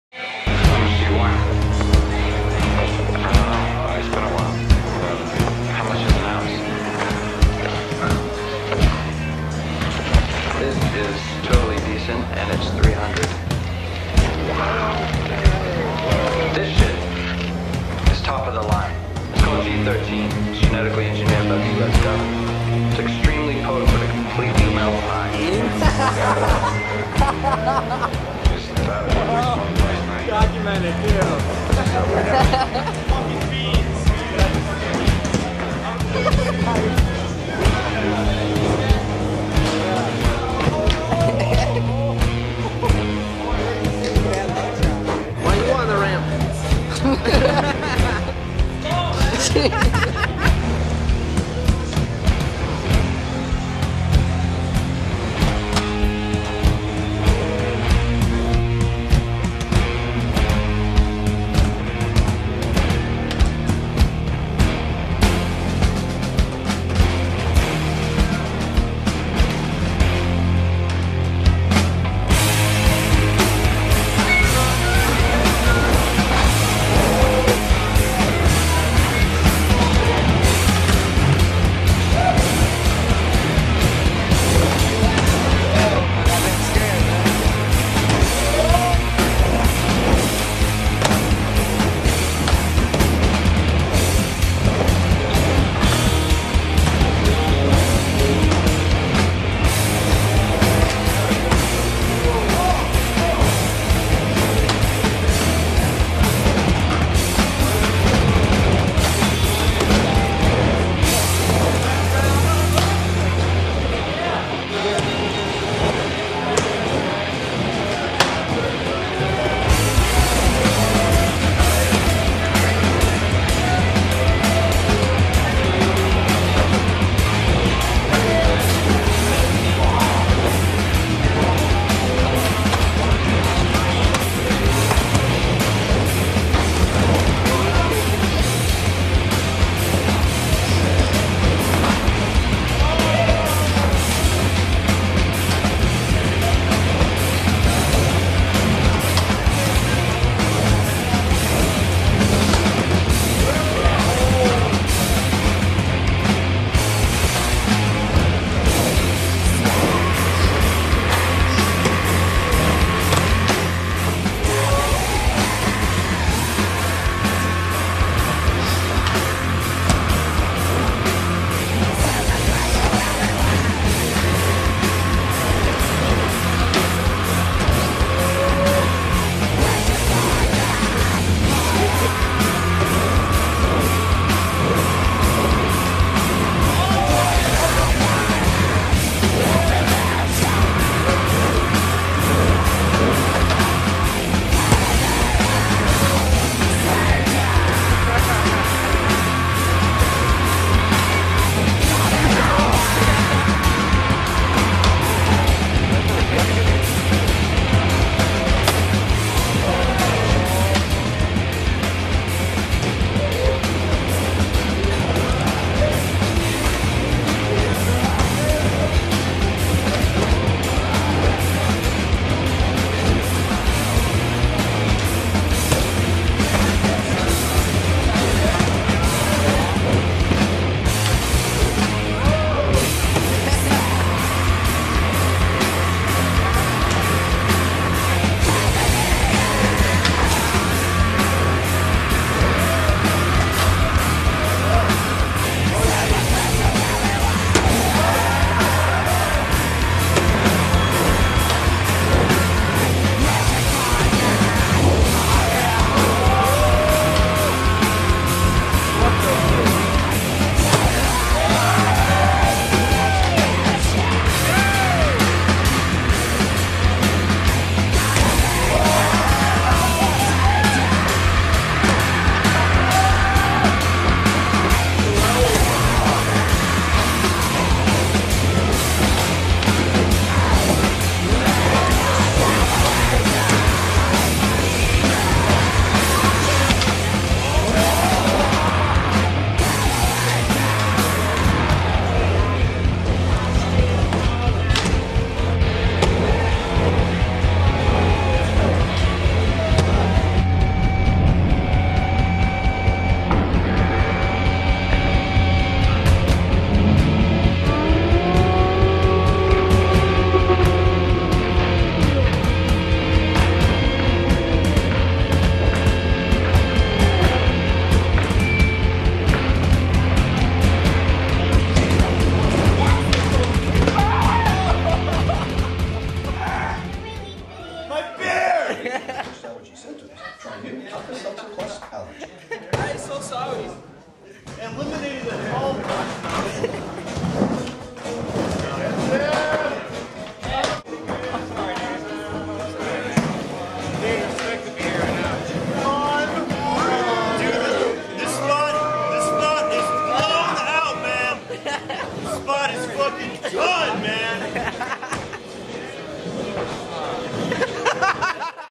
Coming in Fakie at the Beerics
A Beerics Session with a bunch of homies, pete grannis, nick corrigan, kyle kingsmill, david alstadt, G, and more...

filmed by jon Schmoldt